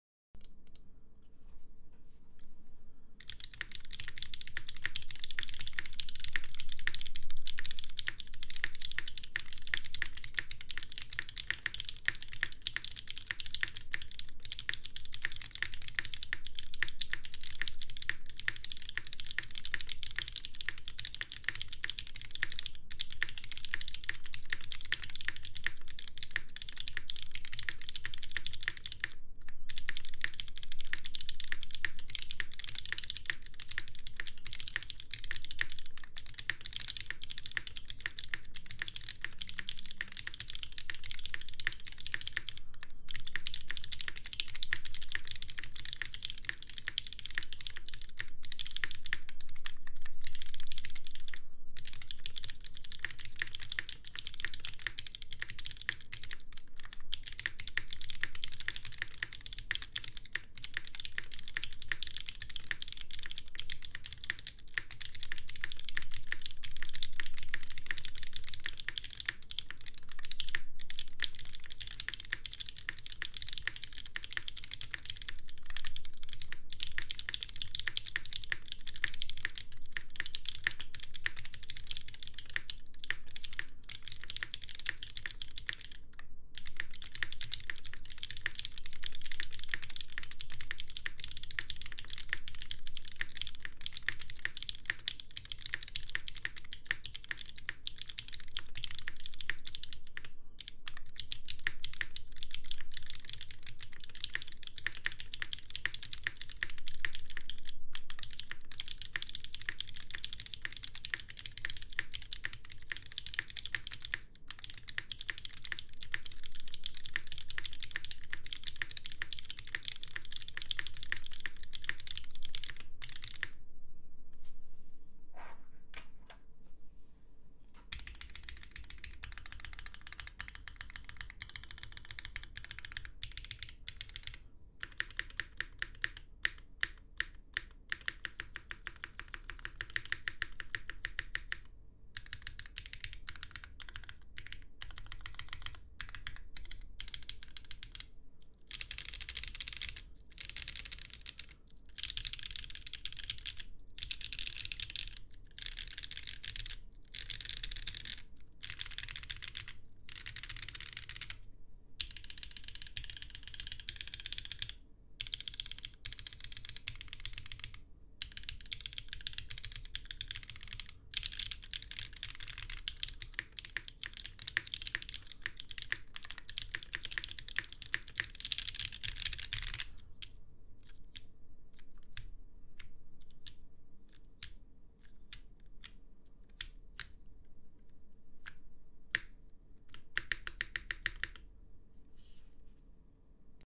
Qneko | Full Alu | 3mill Mx Red | GMK Hammerhead dark
Keyboard: Qneko (Gummy o ring 60 PC by friend)
Plate: Full alu
Mount:  Gummy o ring 40a
Keycaps: Gmk Hammerhead dark
Switches:  3 million actuated Mx red lubed with 205g0
Springs: 55g M2 Sprit
Stabilizers: Cherry clip in lubed with 205g0
Film: 0.15mm Tx
Foam: None
Spacebar foam: None

Wristrest: none
Camera: Fujifilm X-T4
Mic: Lewitt LCT 440 Pure
Deskmat: GMK Pono Pitter Patter (3 stacked)